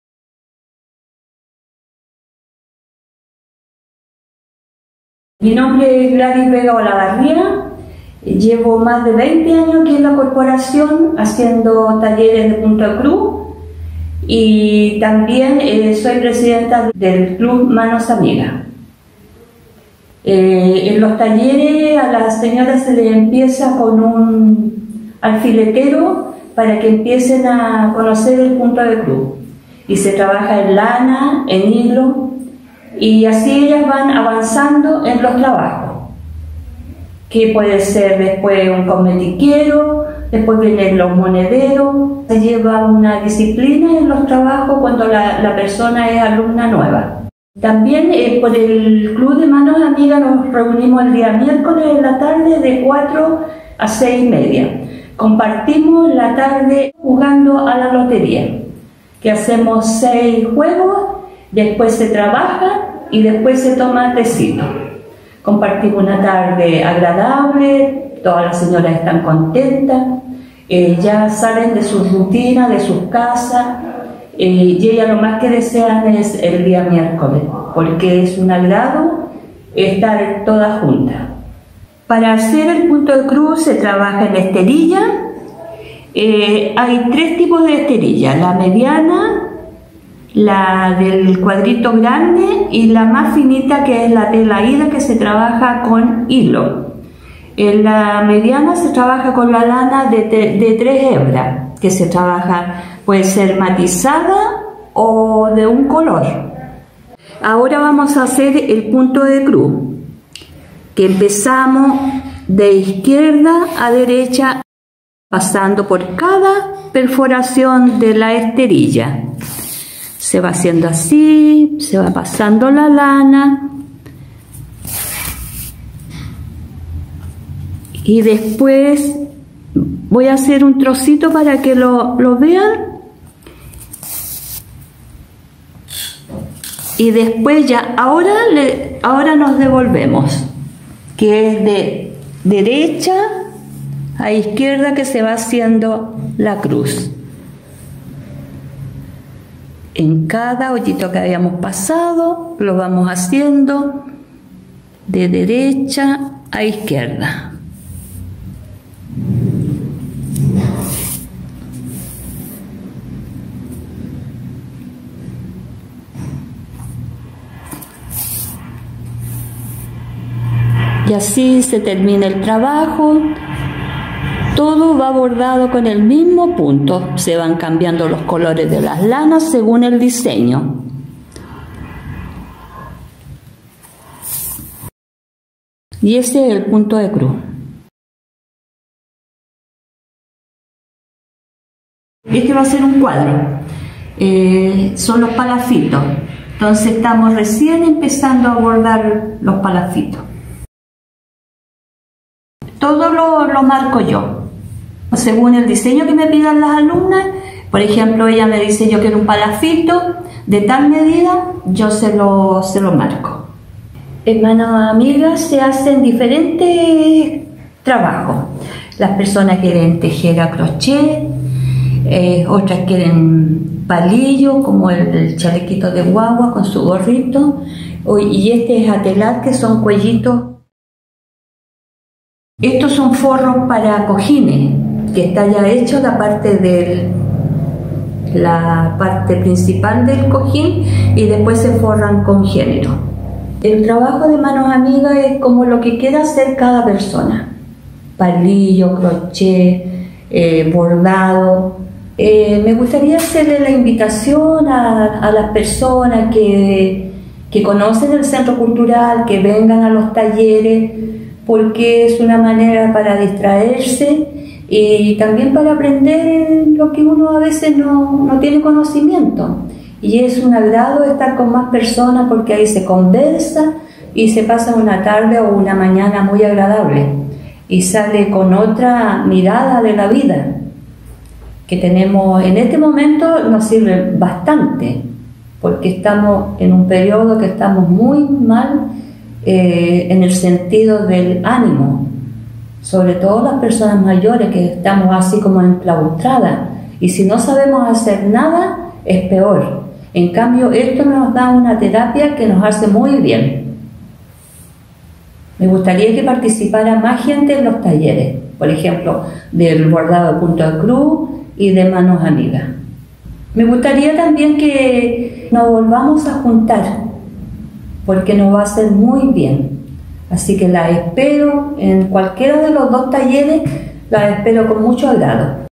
Taller de Bordados: Primera Clase
Corporación Cultural de La Reina
Video Talleres Marzo 2021

Taller de Bordado
Profesora Gladys Vega
Primera Clase: 1 de marzo

Acompáñenos todos los lunes de marzo donde aprenderemos el manejo del color, volumen, y textura, además de los puntos básicos para el bordado con distintos materiales. Asimismo se abordará la técnica de esterillas y sus puntadas con el fin de  bordar y confeccionar tapices, alfombras, entre otros artículos.